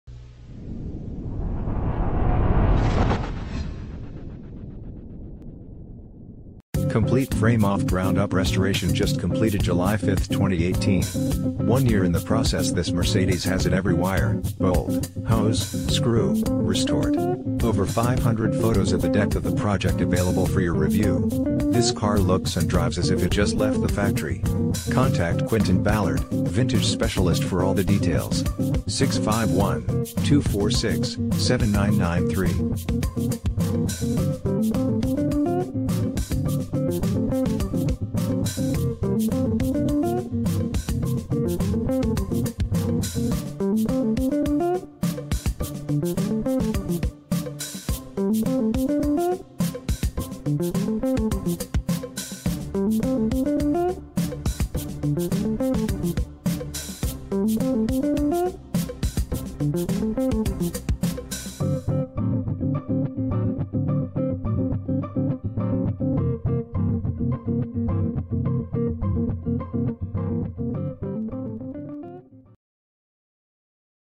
1960 MERCEDES-BENZ 190SL Minneapolis, Golden Valley, St Paul, MN P5846
Complete frame off ground up restoration just completed 7/5/2018. 1 year in the process this Mercedes has had every wire,  bolt, hose, screw, restored. Over 500 photos of the depth of the project available for your review. This car looks and drives as if it just left the factory. Contact Quinton Ballard, Vintage specialist for all the details. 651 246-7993